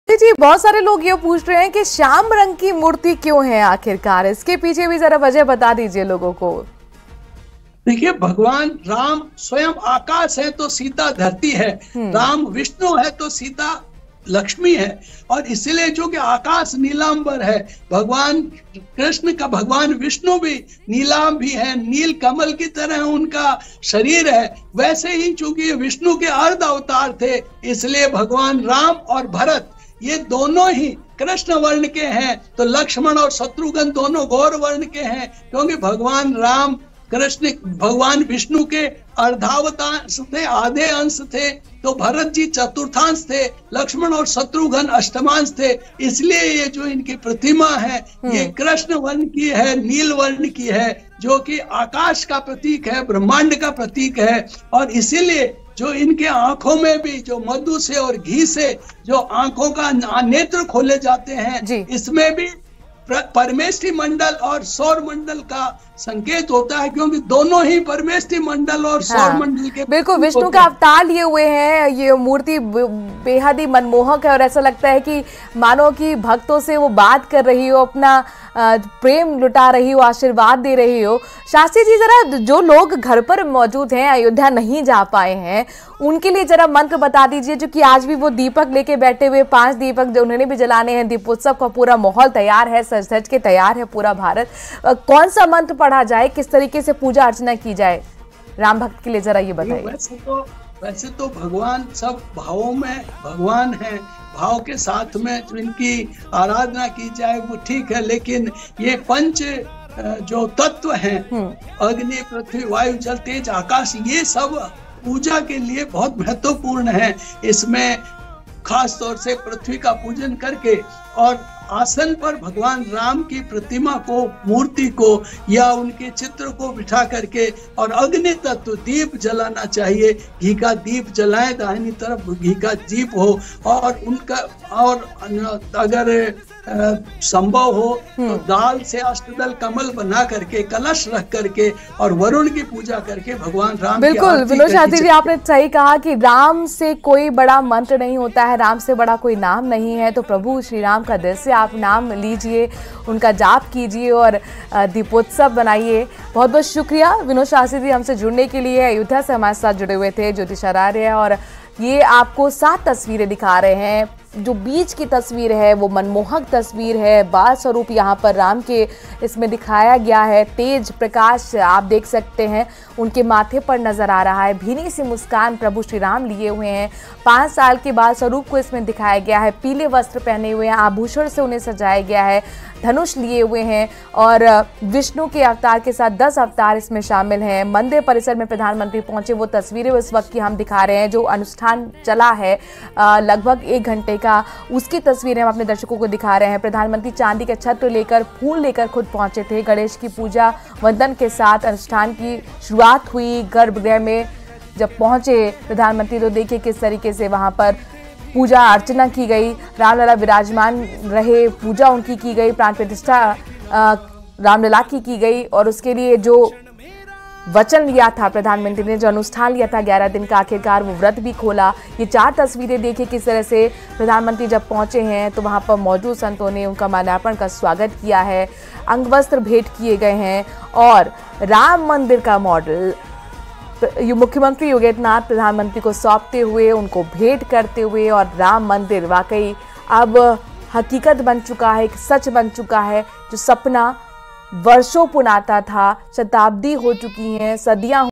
Ram Mandir Pran Pratishtha : राम मंदिर प्राण प्रतिष्ठा में पहुंचने वाला मौलाना कौन? | Breaking News
Ram Mandir Pran Pratishtha : राम मंदिर प्राण प्रतिष्ठा में पहुंचने वाला मौलाना कौन? | Breaking News | PM Modi | Latest Hindi News | Top News

PM Narendra Modi ने आज 11 दिनों के कठोर अनुष्ठान के बाद Ayodhya Ram Mandir की Pran Pratishtha की. इस दौरान PM Narendra Modi ने रामलला की आरती उतारी...